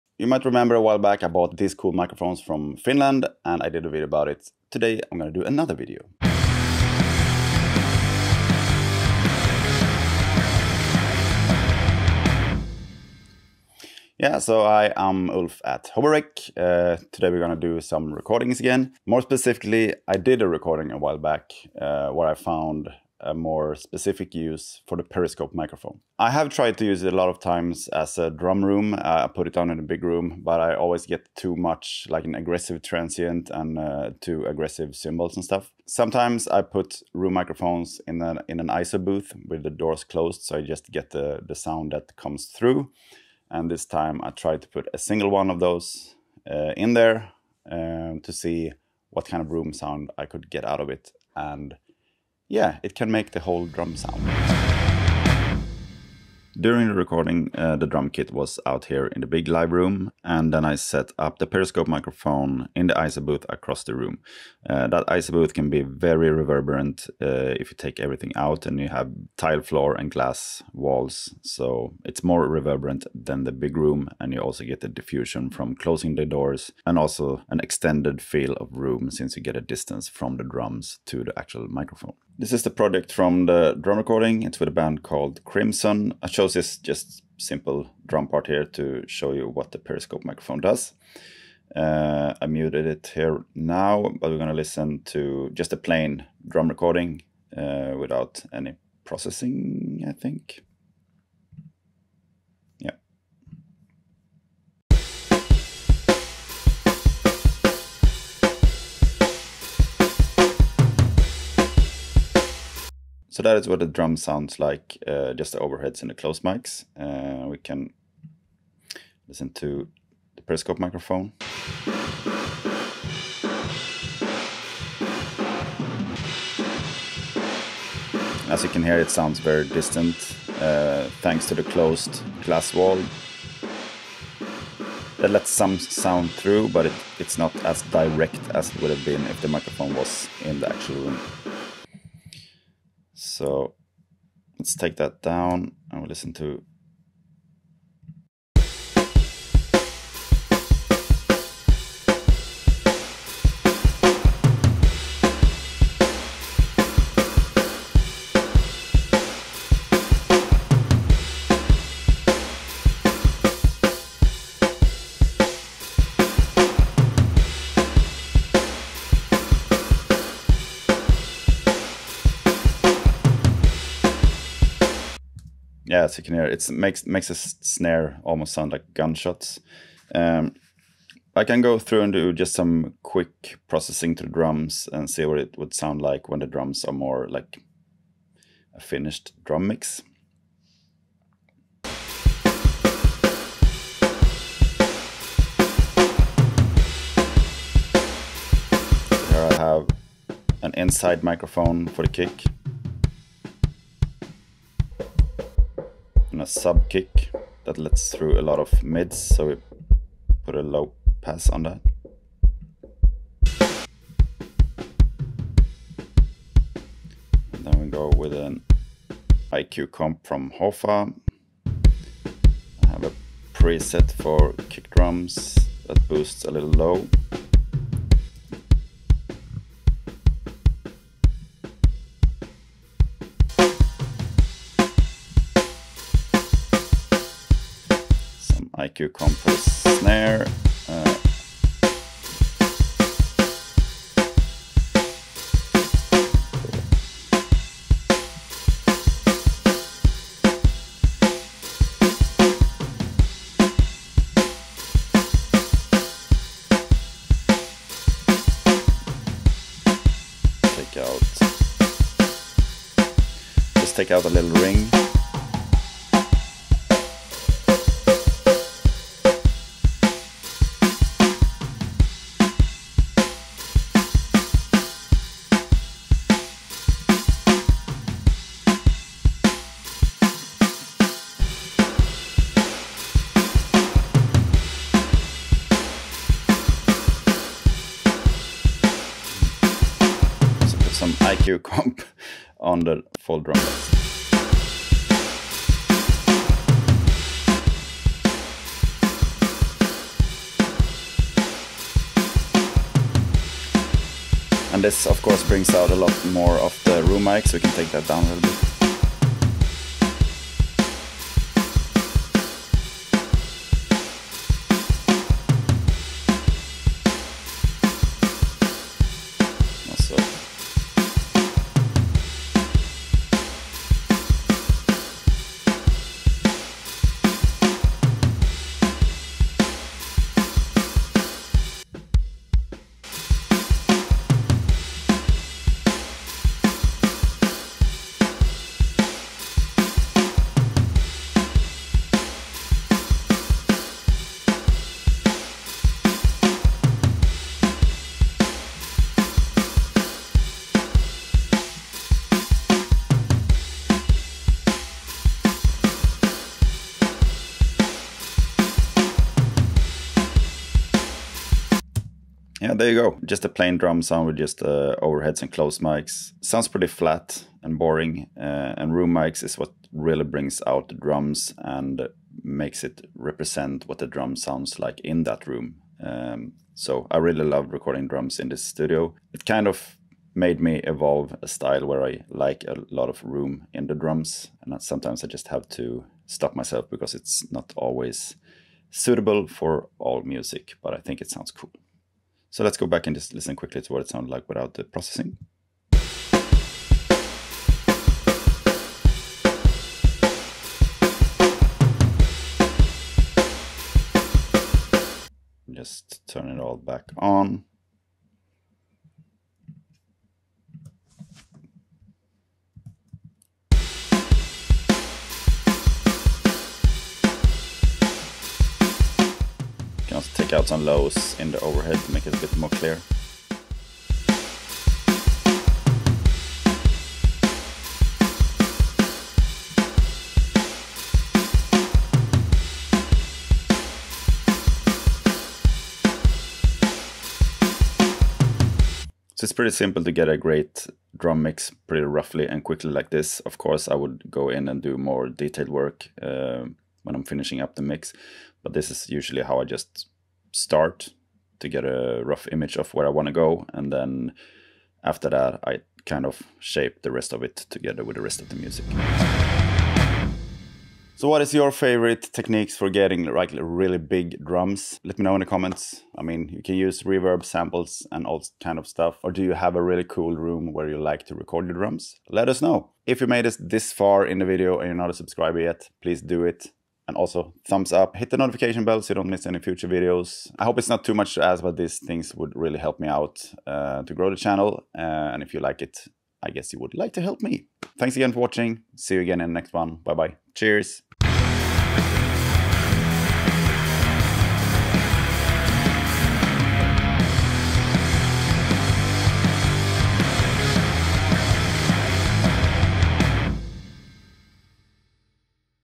Massive Drum Sound With Periscope
Using one Periscope microphone as a room mic to create a massive drum sound.

Check out my drum recording class on Skillshare ( two weeks for free through my link if you're new):